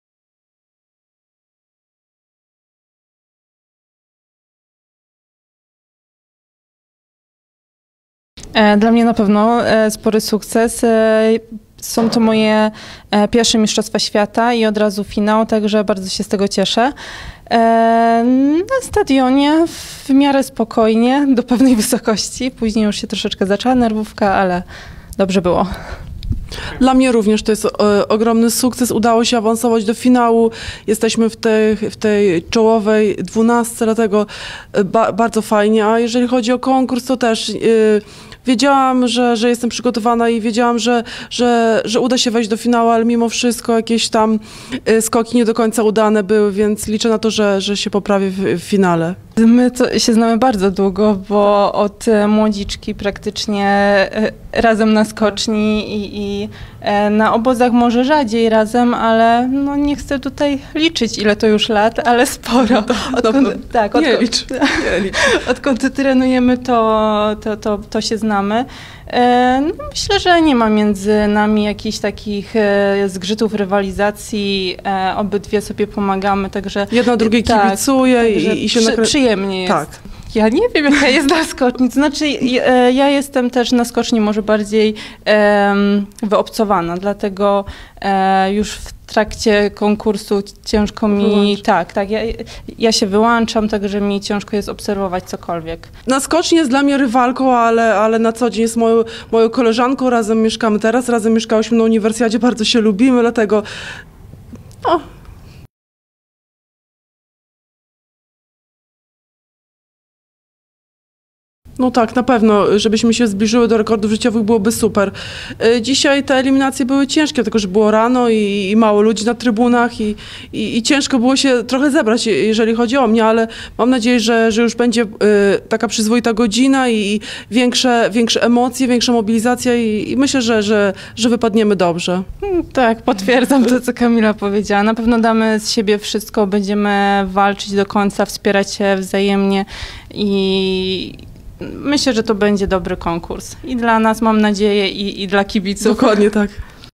Kasprzycka i Stepaniuk po eliminacjach skoku wzwyż
Justyna Kasprzycka i Kamila Stepaniuk skoczyły po 1.92 i awansowały do finału MŚ w Moskwie